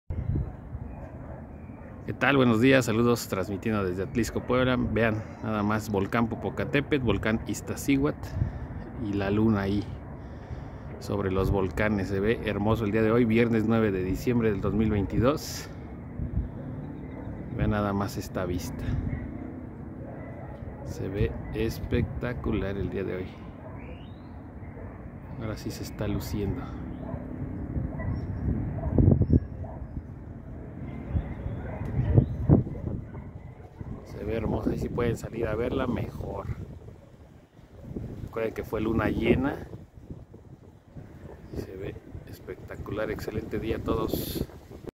La luna y el volcán Popocatépetl 🌋 9 de diciembre del 2022
La luna y el volcán Popocatépetl 🌋 9 de diciembre del 2022 hermoso amanecer